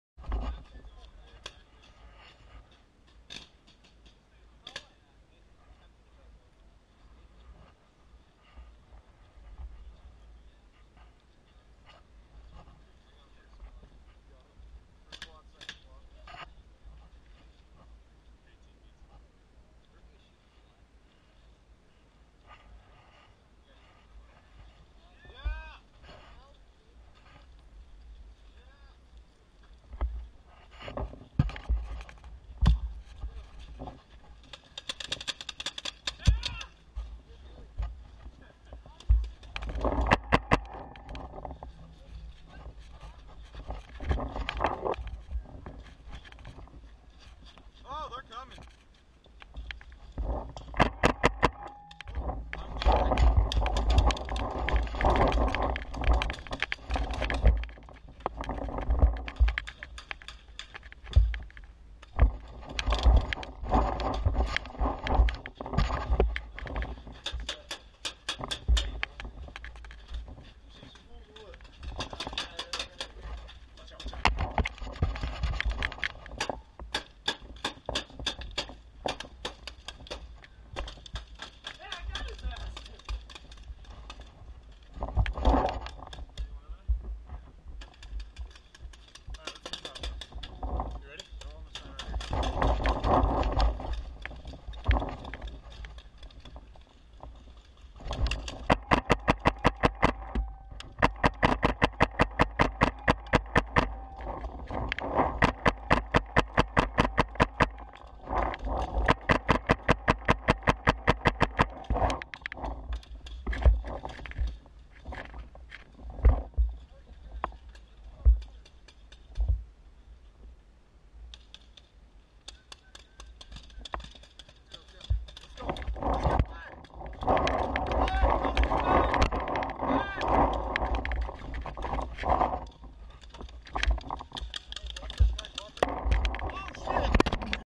The University High School football team's trip to Blitz Paintball. It's paintball from the ...
Original Date: Jul 18th, 2017

The University High School football team's trip to Blitz Paintball. It's paintball from the paintball gun's perspective.

Original Key: ntfcIY5V